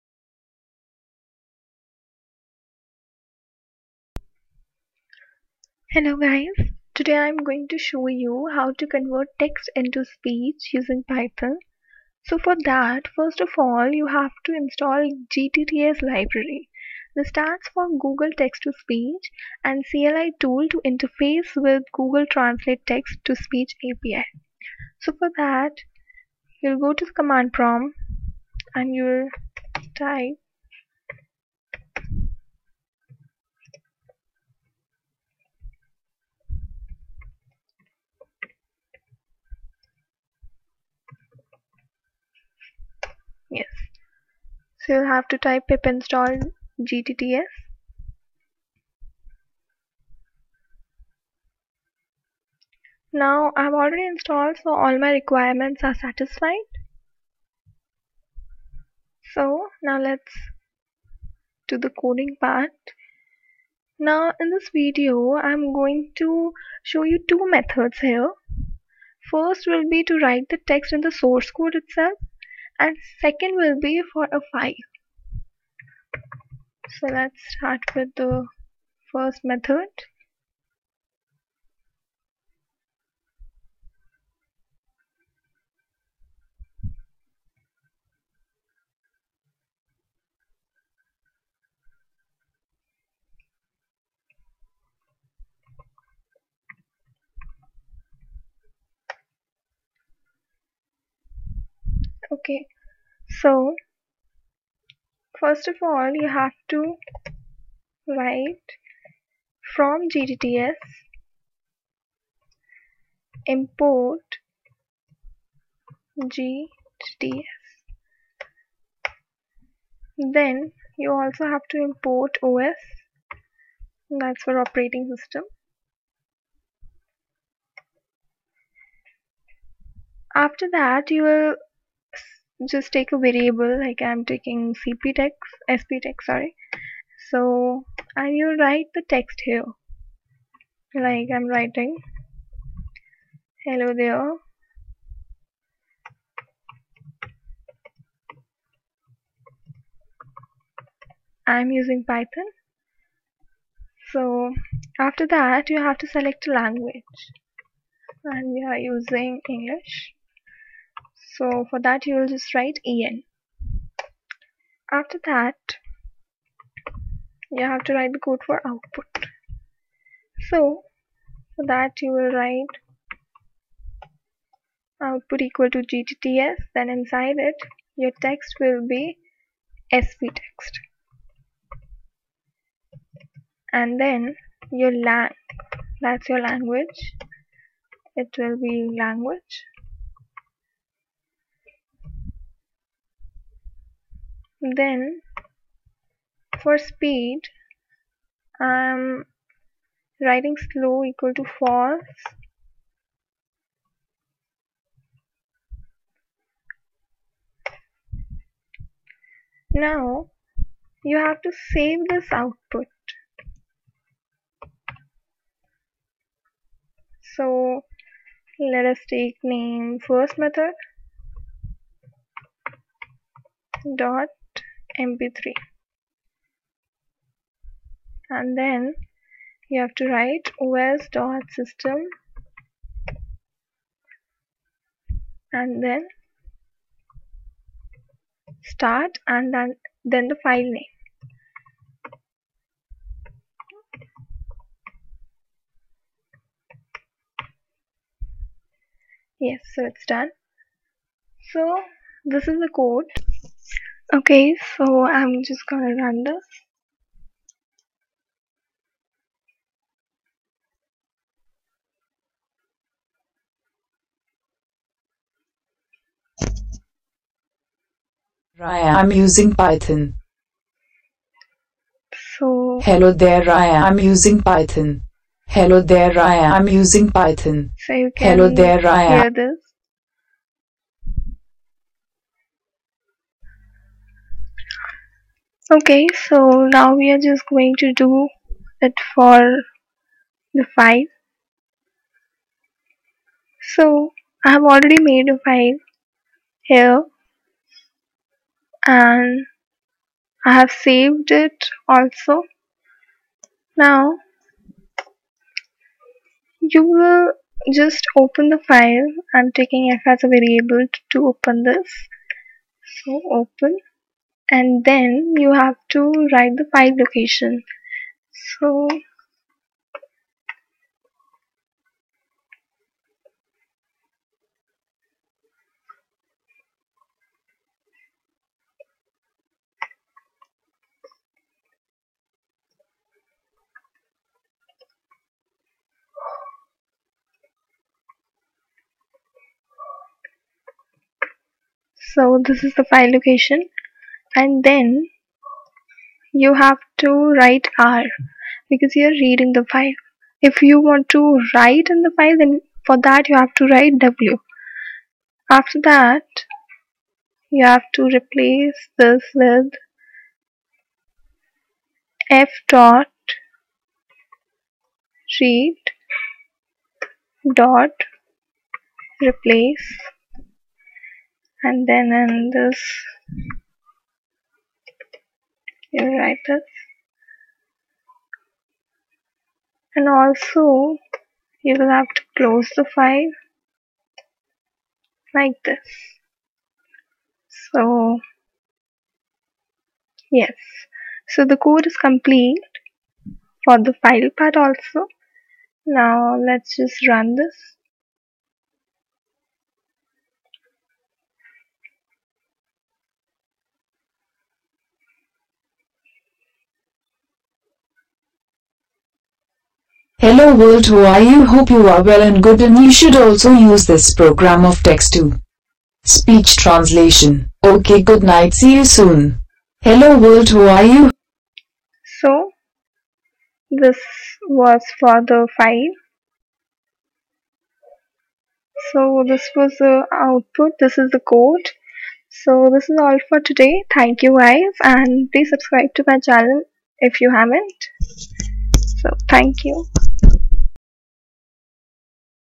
Text To Speech | Python | gTTS | Two Methods
Hey everyone this video is to show you how you can convert text to speech using python. And in this video I have used gtts library that is google text to speech. I have explained two methods for this.